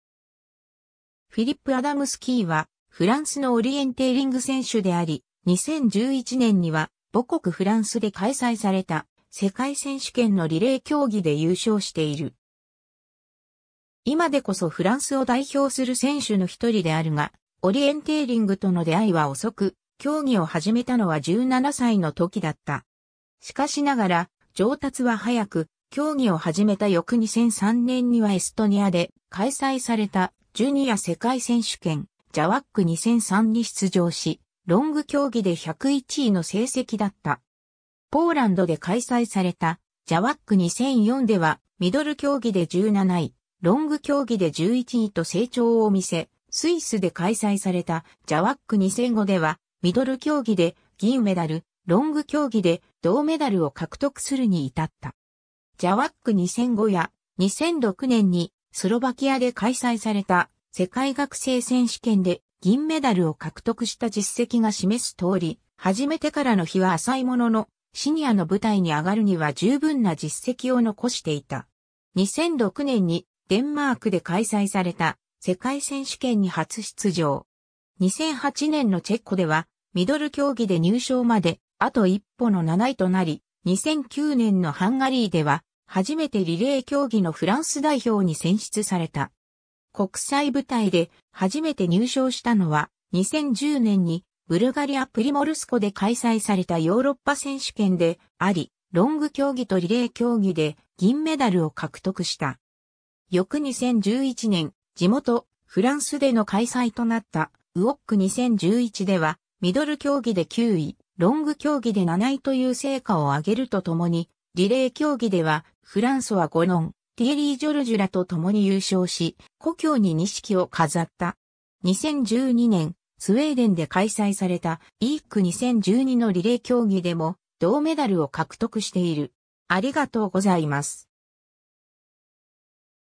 フィリップ・アダムスキー
フィリップ・アダムスキー（Philippe Adamski、1985年4月8日 - ）はフランスのオリエンテーリング選手であり、2011年には母国フランスで開催された世界選手権のリレー競技で優勝している。
 今でこそフランスを代表する選手の一人であるが、オリエンテーリングとの出会いは遅く、競技を始めたのは17歳の時だった。
しかしながら上達は速く、競技を始めた翌2003年にはエストニアで開催されたジュニア世界選手権、JWOC2003に出場し、ロング競技で101位の成績だった。
ポーランドで開催されたJWOC2004ではミドル競技で17位、ロング競技で11位と成長を見せ、スイスで開催されたJWOC2005ではミドル競技で銀メダル、ロング競技で銅メダルを獲得するに至った。
 JWOC2005や、2006年にスロバキアで開催された世界学生選手権（WUOC）で銀メダルを獲得した実績が示す通り、始めてからの日は浅いものの、シニアの舞台に上がるには充分な実績を残していた。
 2006年にデンマークで開催された世界選手権（WOC）に初出場。
2008年のチェコではミドル競技で入賞まであと一歩の7位となり、2009年のハンガリーでは初めてリレー競技のフランス代表に選出された。
 国際舞台で初めて入賞したのは、2010年にブルガリア・プリモルスコで開催されたヨーロッパ選手権（EOC）であり、ロング競技とリレー競技で銀メダルを獲得した。
 翌2011年、地元フランスでの開催となったWOC2011ではミドル競技で9位、ロング競技で7位という成果をあげるとともに、リレー競技ではフランソワ・ゴノン、ティエリー・ジョルジュらと共に優勝し、故郷に錦を飾った。
 2012年、スウェーデンで開催されたEOC2012のリレー競技でも銅メダルを獲得している。